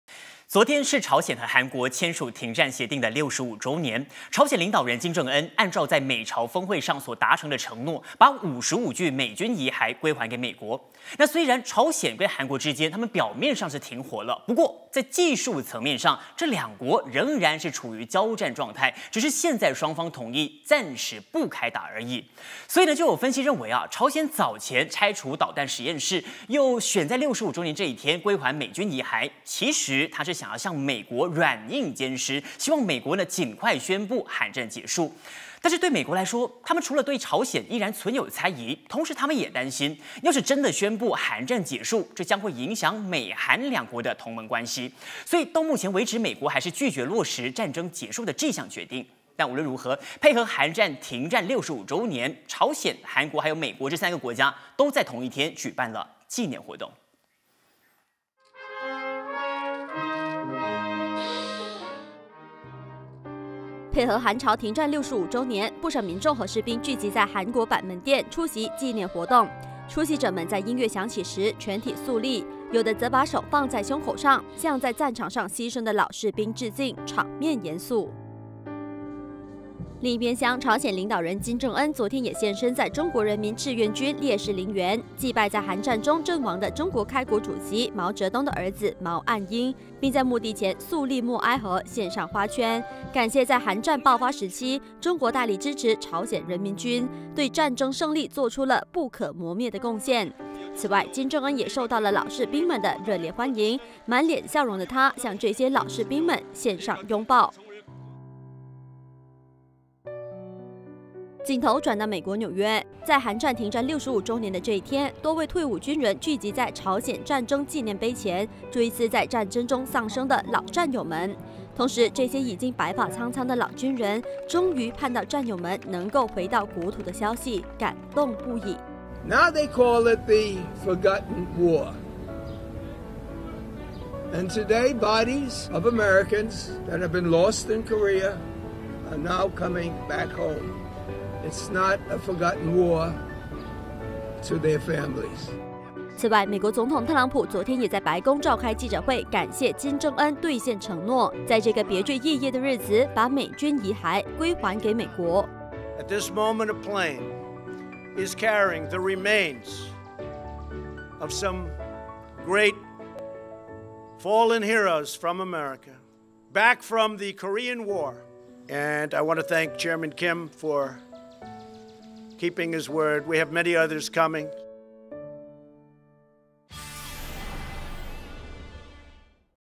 Prime Talk 八点最热报 28/07/18 - 朝鲜归还55具美军遗骸
昨天是韩朝签署《停战协定》65周年，朝鲜领导人金正恩按照在美朝峰会上所达成的承诺，将55具美军遗骸归还给美国。特朗普也在记者会上，感谢金正恩愿意归还美军遗骸。